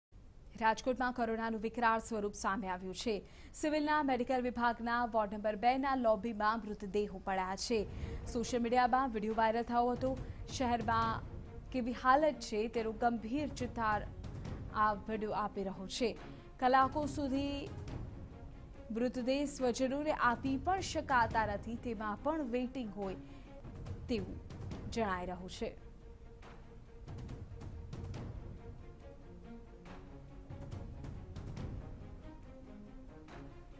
Viral video shows dead bodies lying in lobby area of Rajkot Civil hospital | TV9News
Viral video shows dead bodies lying in lobby area of Rajkot Civil hospital | TV9News | D30